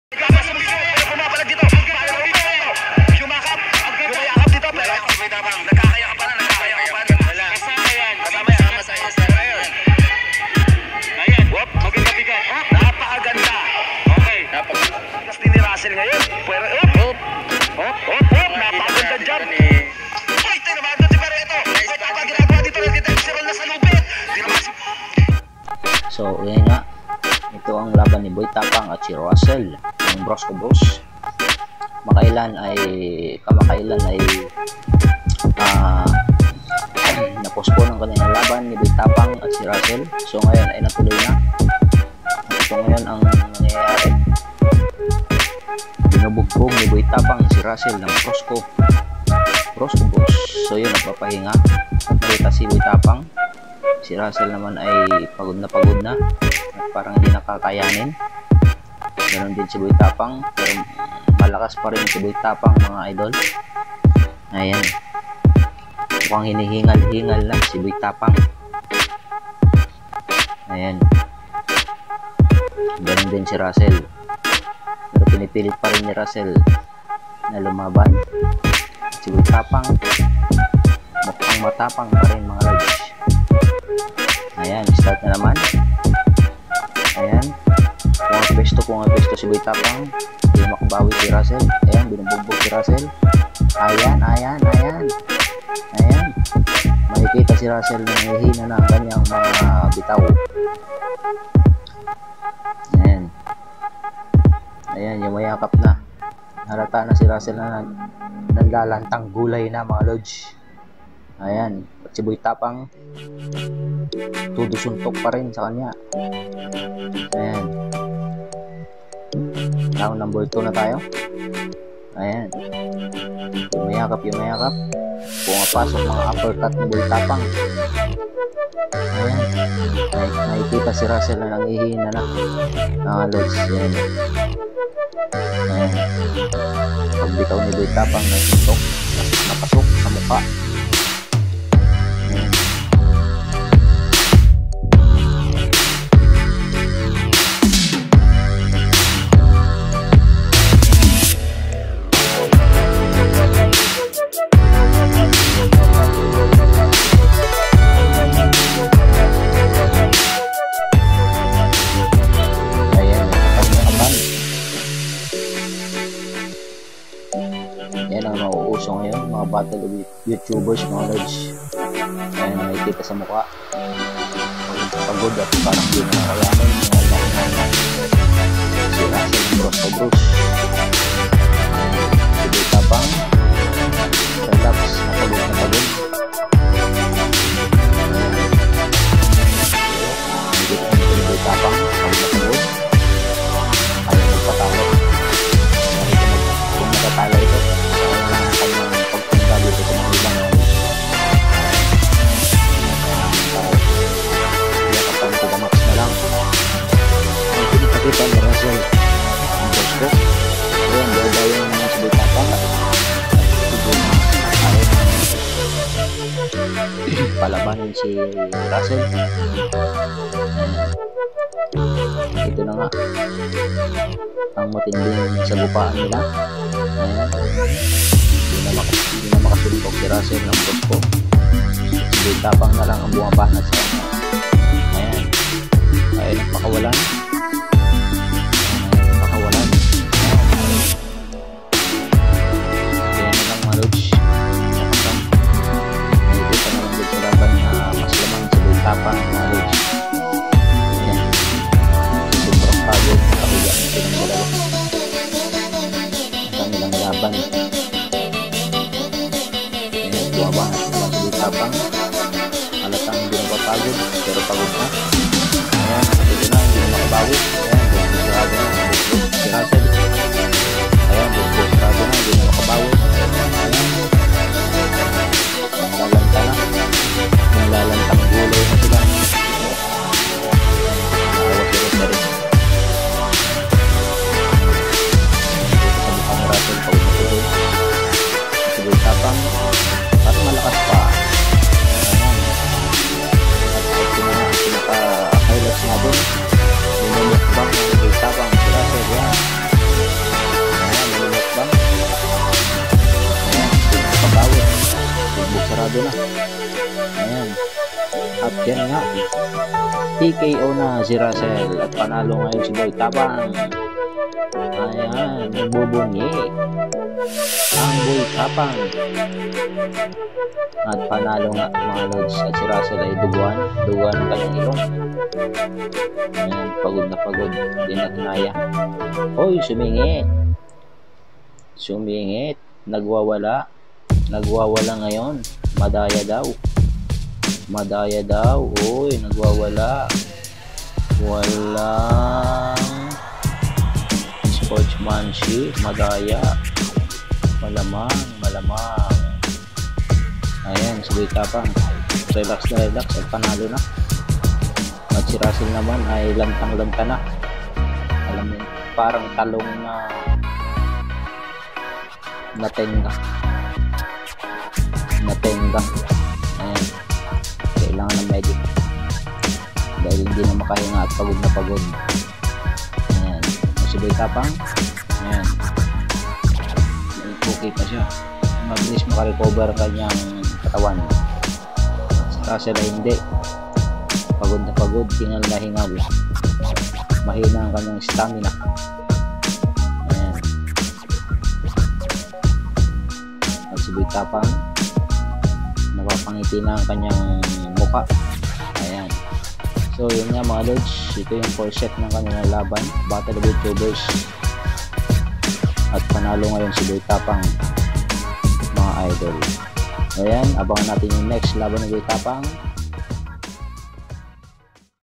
Highlights sa laban ni BOYTAPANG AT RUSSELL NG BROSKOBROSS SA BATTLE OF YOUTUBERS BOY TAPANG WIN
Shout out sa mga katapang dyan abangan ang susunod na laban ni boy tapang